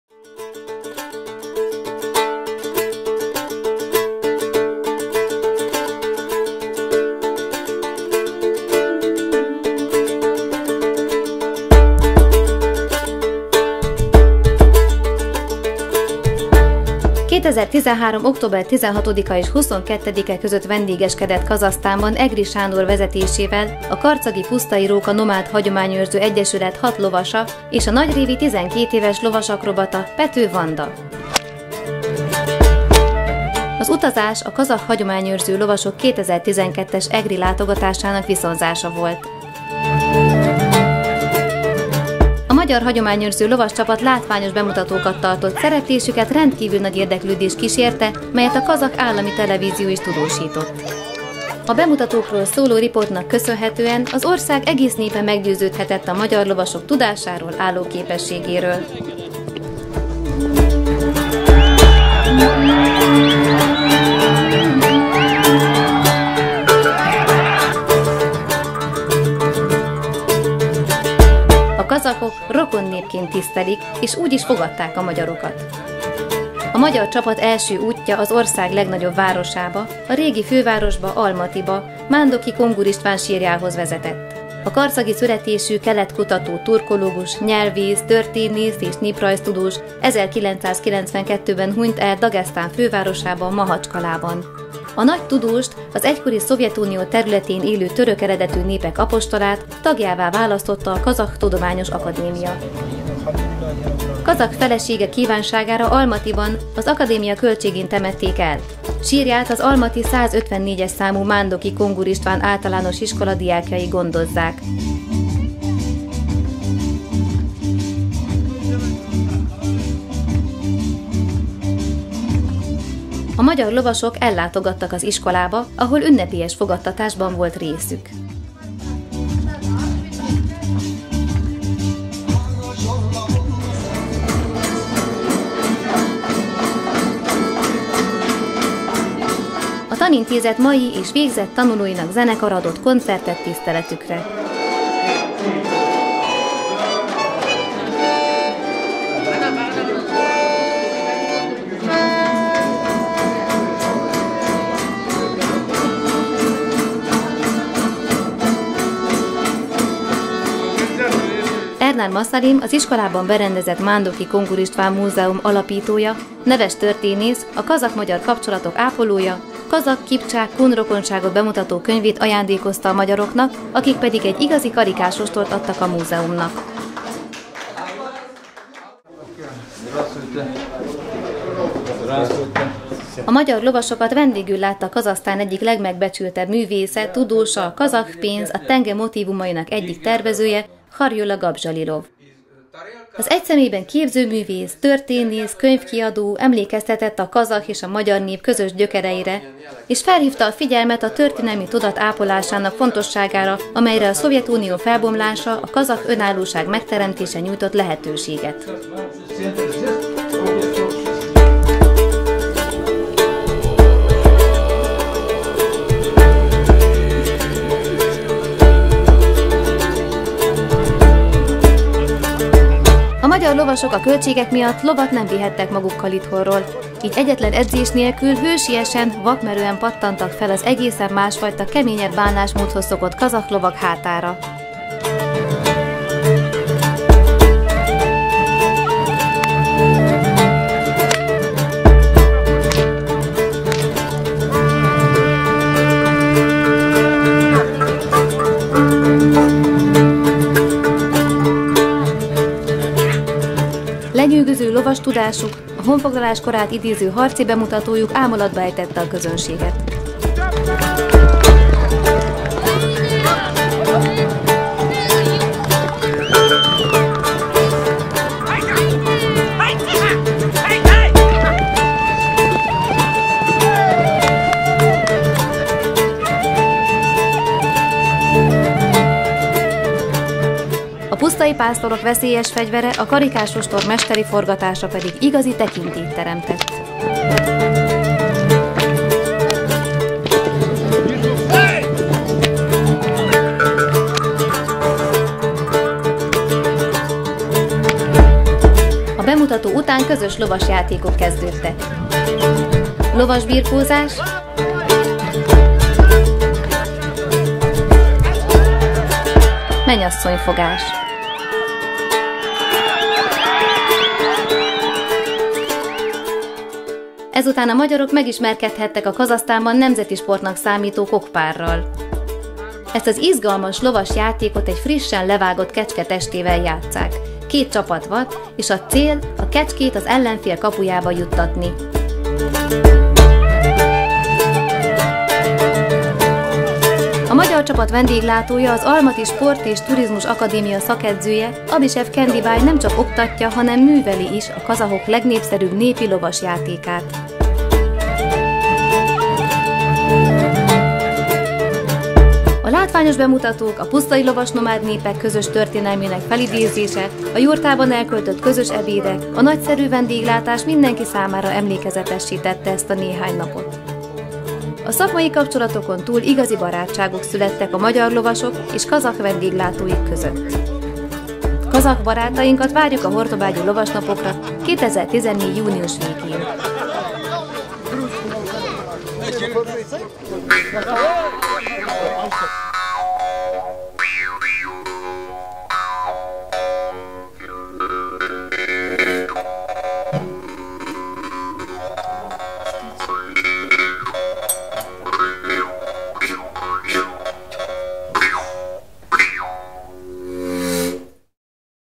Magyar zászló a kazah sztyeppén
Ez a film egy rövid összeállítás a karcagi Pusztai Róka Nomád Hagyományőrző Egyesület Egri Sándor által szervezett 2013-as kazahsztáni útjáról.